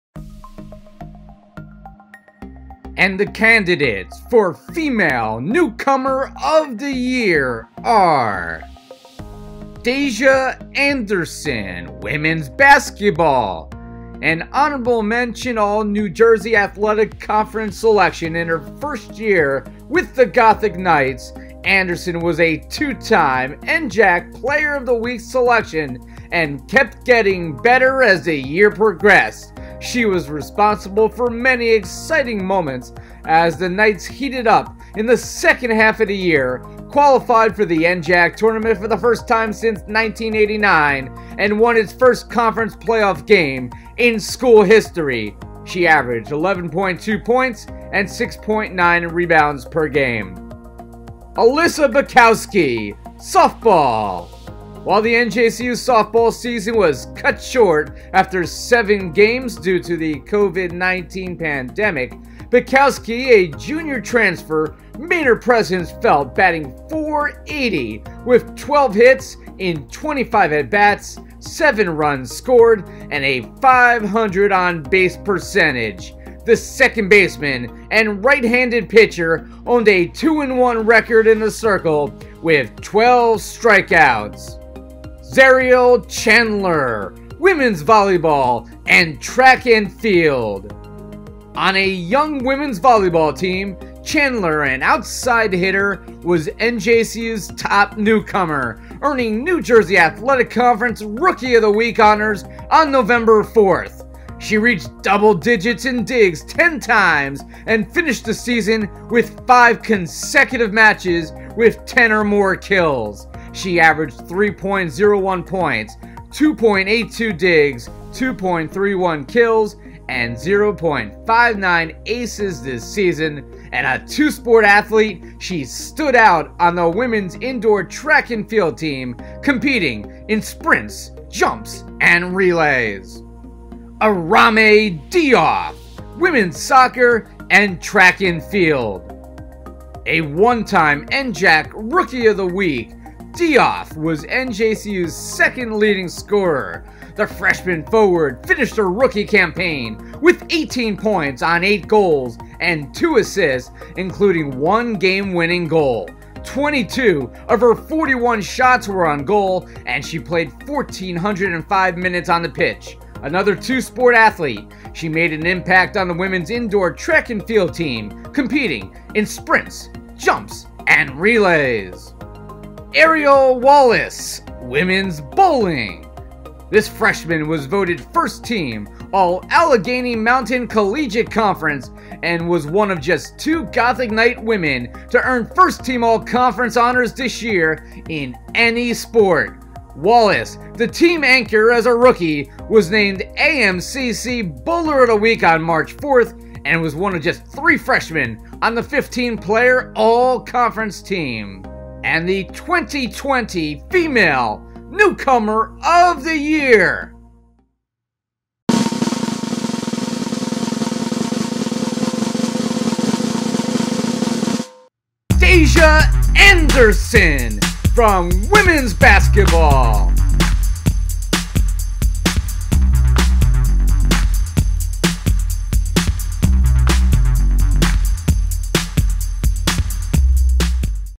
2020 The Gothics---Female Newcomer of the Year
On day 15 of the 2020 Gothics, NJCU Athletics held a watch party to announce its Male and Female Newcomers of the Year.

The four finalists for Female Newcomer of the Year are:
•Dayjah Anderson, Women's Basketball
•Alissa Bikowski, Softball
•Xarial Chandler, Women's Volleyball/Indoor Track & Field
•Arame Diouf, Women's Soccer/Indoor Track & Field
•Arielle Wallace, Women's Bowling